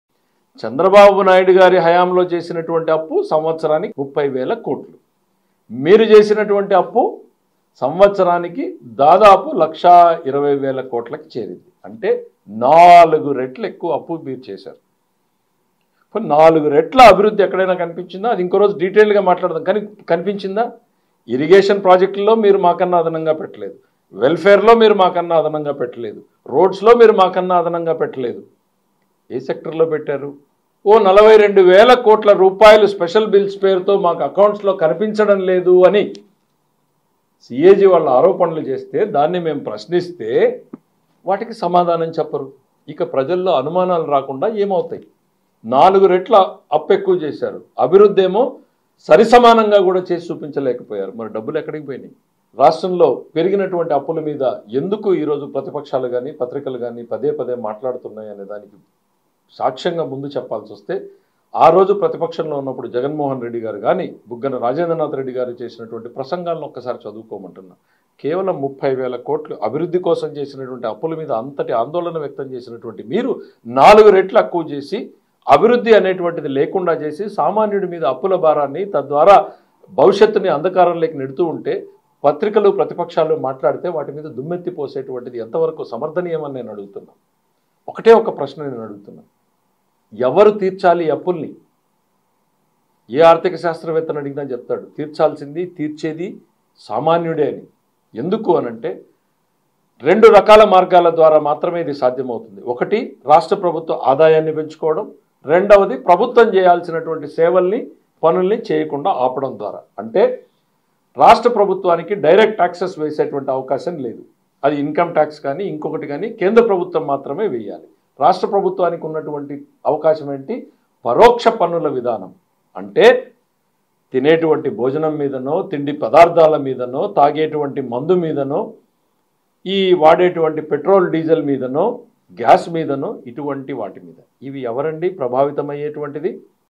తెలుగుదేశం కంటే 4 రెట్లు ఎక్కువ అప్పు చేసారు.మరి నాలుగో వంతు అభివృద్ధి అయినా చేయలేదేం-పయ్యావుల కేశవ్
ఏపీలోని ప్రతి ఒక్కరు తప్పక చూడాల్సిన ఈ వీడియోలో... ప్రస్తుత రాష్ట్ర ఆర్థికపరిస్థితి, రానున్న ఆర్థిక కష్టాలను తెలుగుదేశం ఎమ్మెల్యే, పీఏసీ చైర్మన్ పయ్యావుల కేశవ్ చాలా స్పష్టంగా వివరించారు. ఈ వాస్తవాలు వింటే అసెంబ్లీలో జగన్ రెడ్డి ఎన్ని అబద్ధాలు చెప్పారో తెలుస్తుంది.